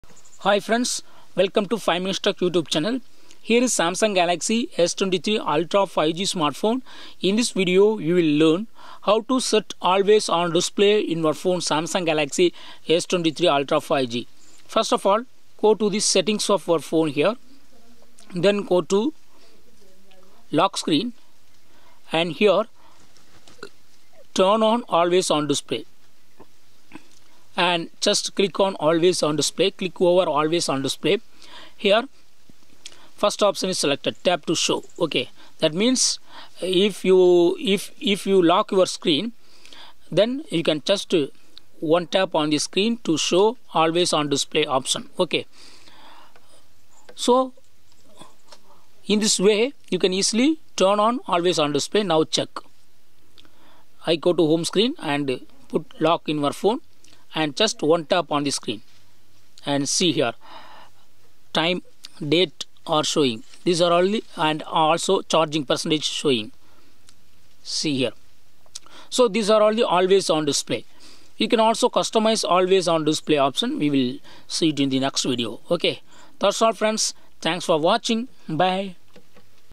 How to set always on display in Samsung Galaxy S23 Ultra 5G
Samsung Galaxy S23 Ultra 5G always on display settings: Learn here how to set always on display in Samsung Galaxy S23 Ultra 5G smartphone.